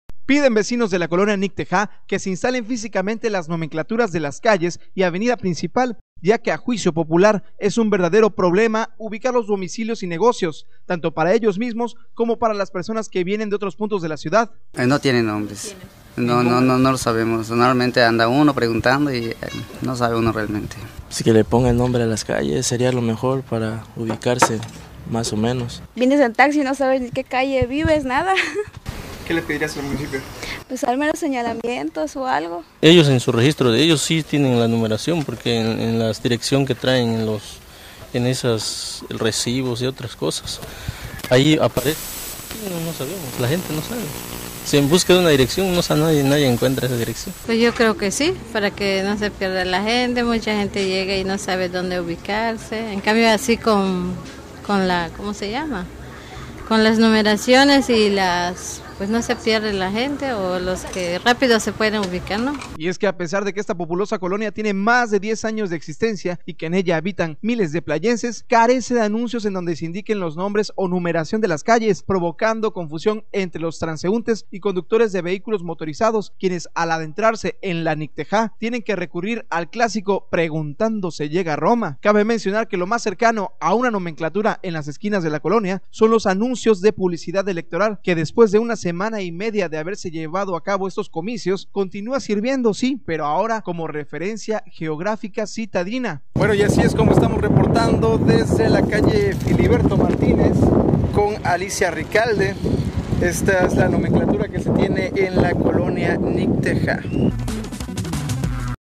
Necesitan nomeclatura en la "Nicte-Ha"
Playa del Carmen, Q. Roo.- Piden vecinos de la colonia Nicte-há que se instalen físicamente las nomenclaturas de las calles y avenida principal ya que a juicio popular es un verdadero problema ubicar los domicilios y negocios; tanto para ellos mismos, como para las personas que vienen de otros puntos de la ciudad. Y es que a pesar de que esta populosa colonia tiene más de 10 años de existencia y que en ella habitan miles de playenses, carece de anuncios en donde se indiquen los nombres o numeración de las calles, provocando confusión entre los transeúntes y conductores de vehículos motorizados quienes al adentrarse en la Nicte-há tienes que recurrir al clasico "preguntando se llega a Roma".